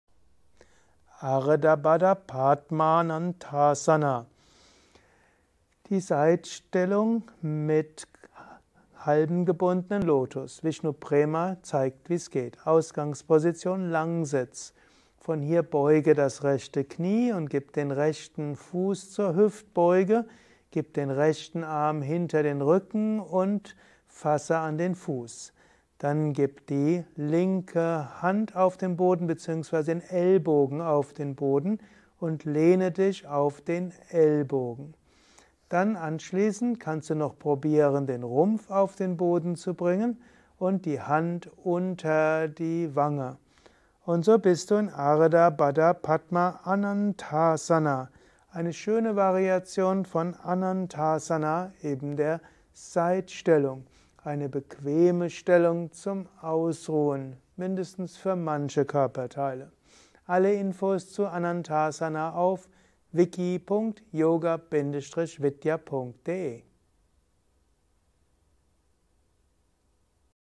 Ardha Baddha Padma Anantasana - Asana Lexikon 56
Ardha Baddha Padma Anantasana bzw. अर्धबद्धपद्मानन्तासन, ardha-baddha-padma-anantāsana: Erläuterung, Vorführung und Übungsanleitung. Ardha Baddha Padma Anantasana gehört im Yoga zu den Rückbeugen, Asanas im Sitzen.

Ardha Baddha Padma Anantasana ist eine Variation von Anantasana